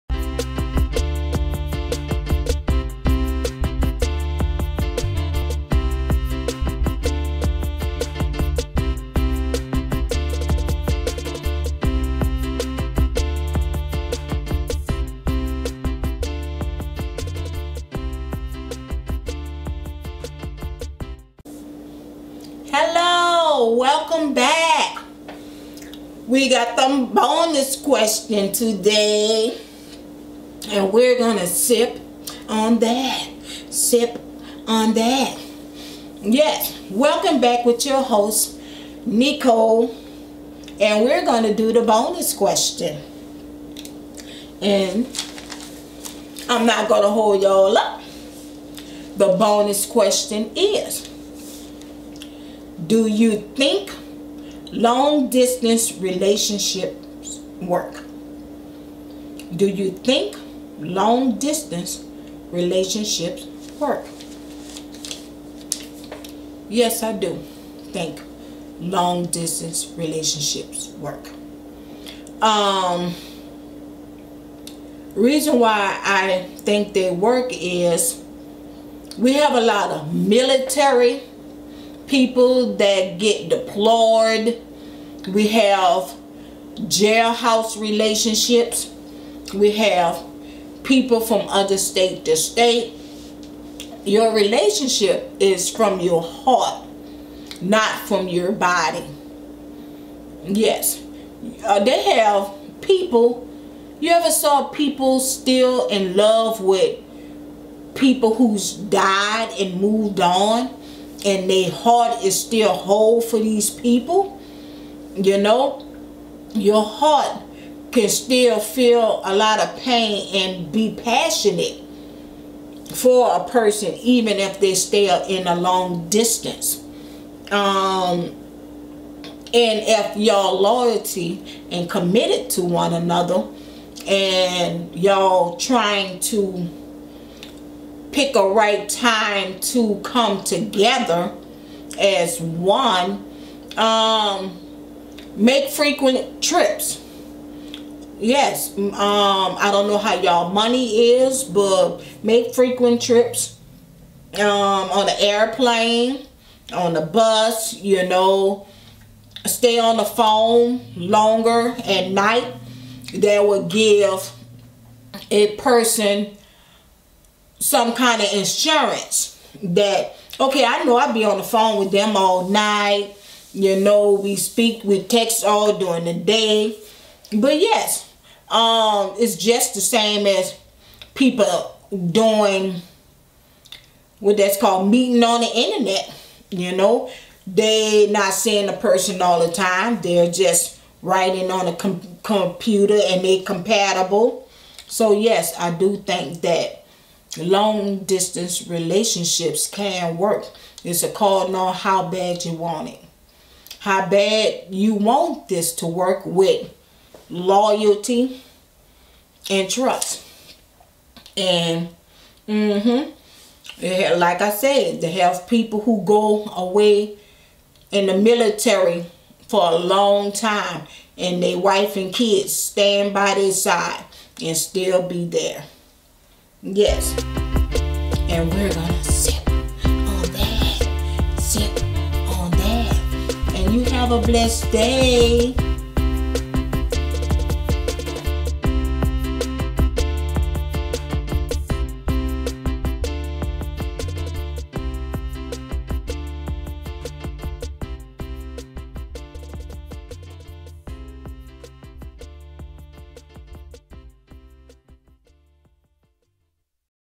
SIPPON THAT WITH NICOLE
BOUNS QUESTION: DO U THINK LONG DISTANCE RELATIONSHIPS WORK ?
THIS IS Q & A THANK YOU SIPPERS